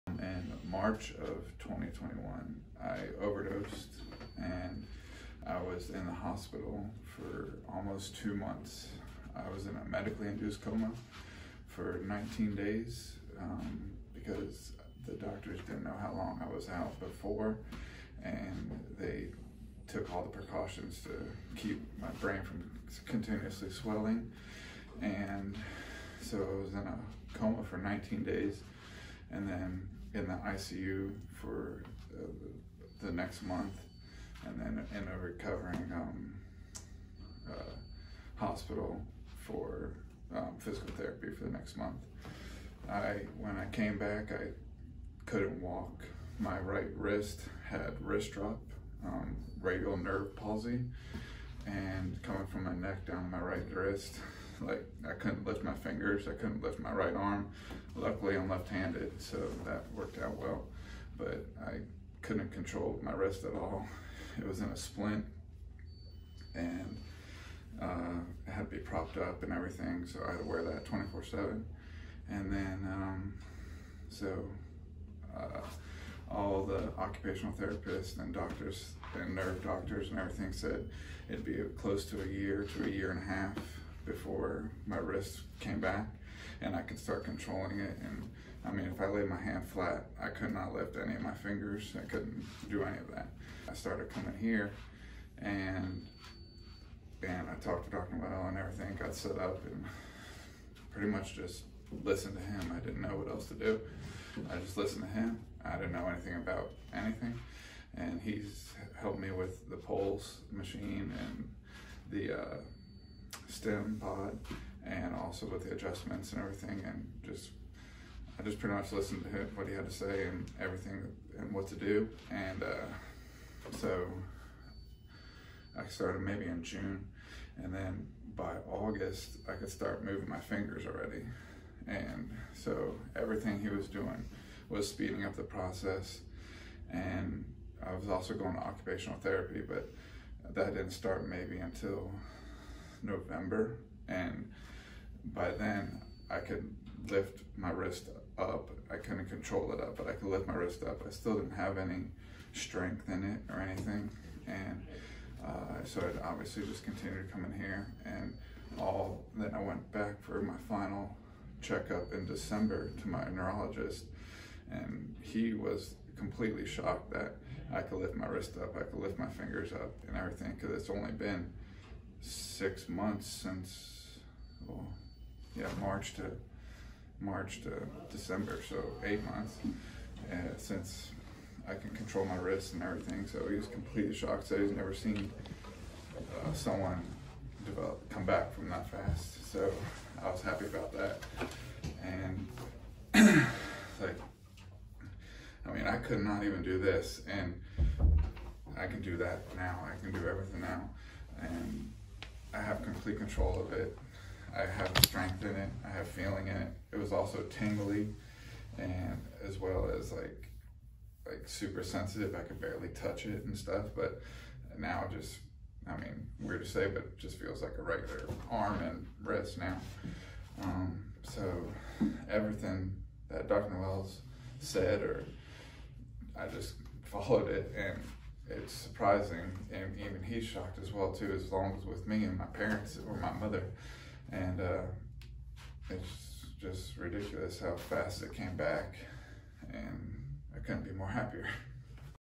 Radial nerve palsy recovery
After waking from 19 days in a coma, his right radial nerve was paralyzed.  We used Pulse PEMF, StimPod and Brain Based applications with the Focus Builder App.  100% function restored!!!